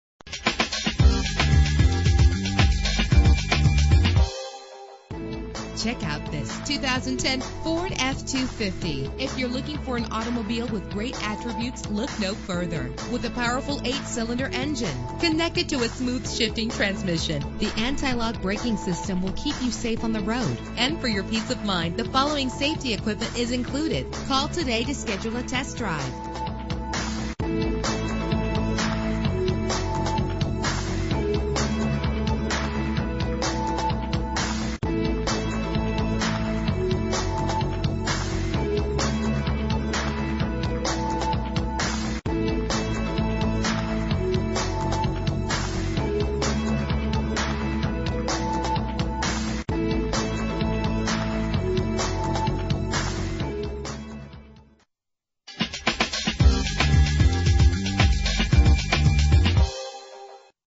2010 Ford F-250 - Mesa AZ
Year: 2010
Make: Ford
Model: F-250
Trim: Super Duty
Engine: 6.4 liter 8 cylinder 32 valve
Transmission:
Color: Ingot Silver
Mileage: 0
Address:
460 E Auto Center Dr